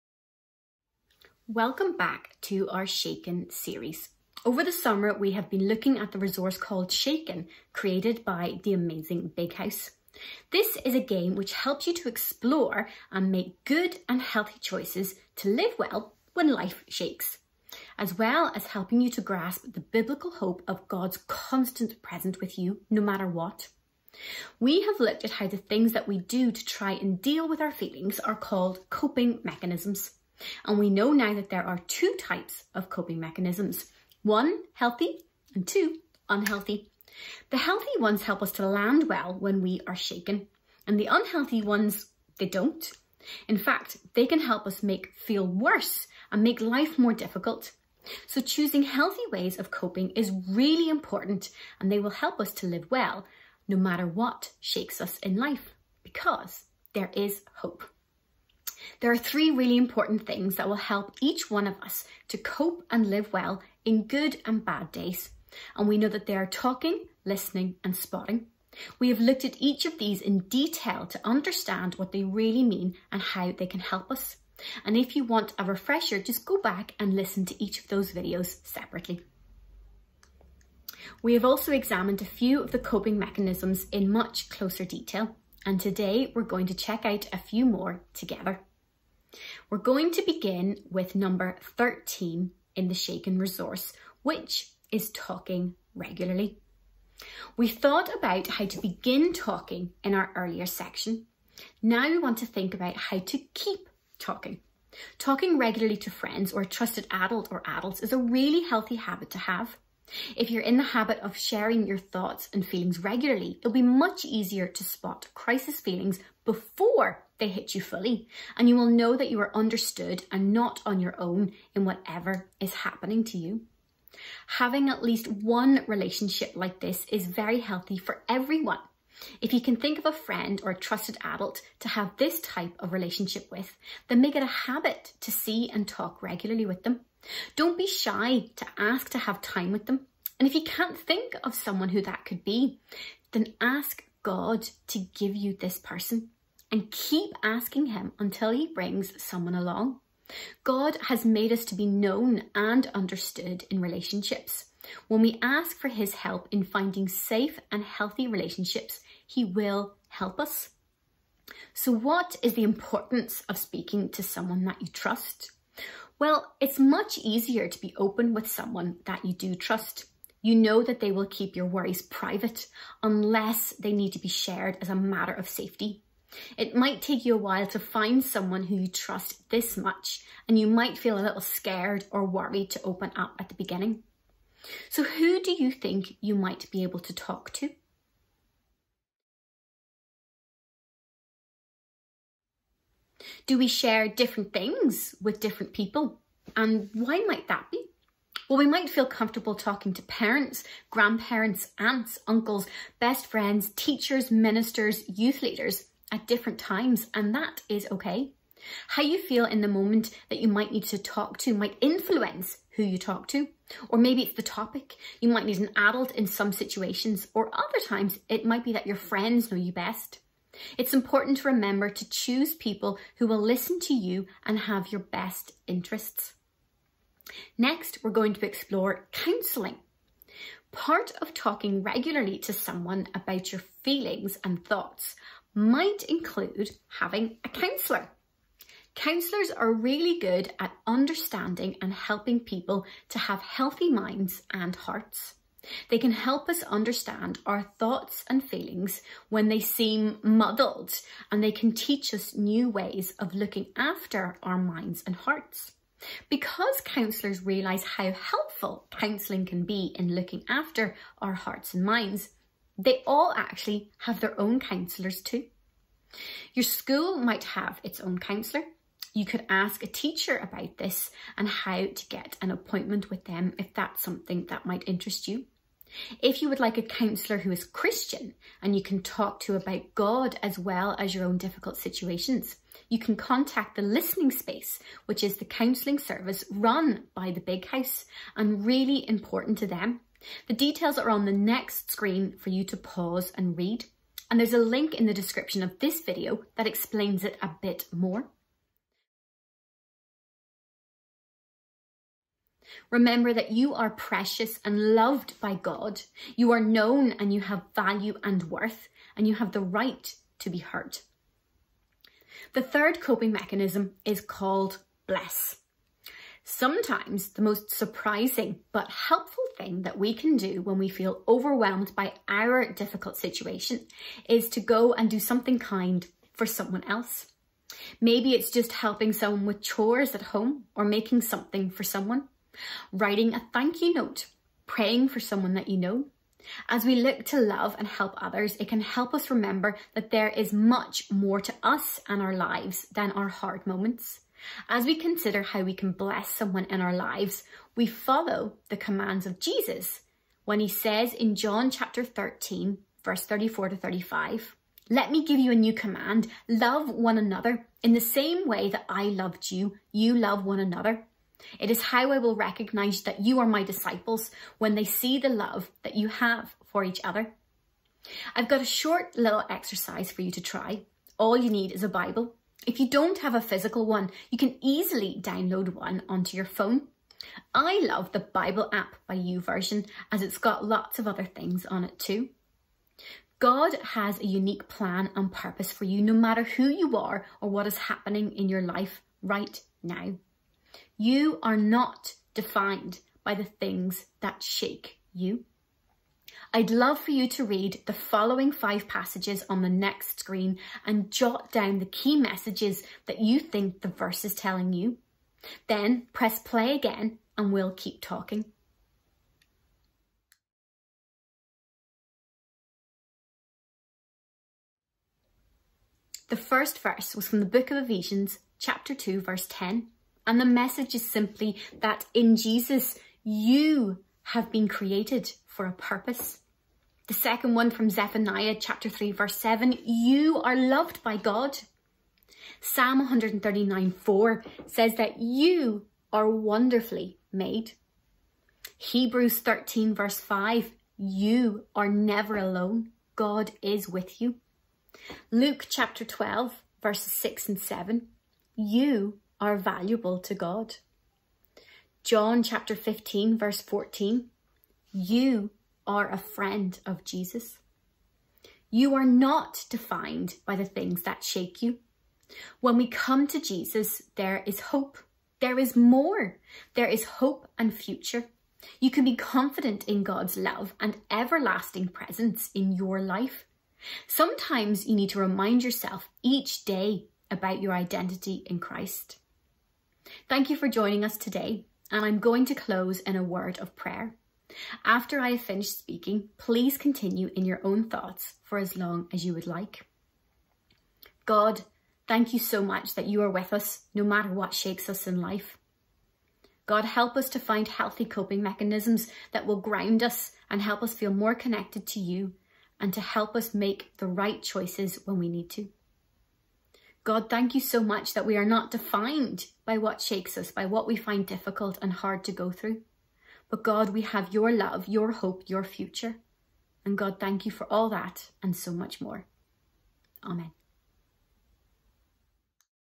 Shaken 7
We look at a few more coping strategies from The Big House resource called Shaken.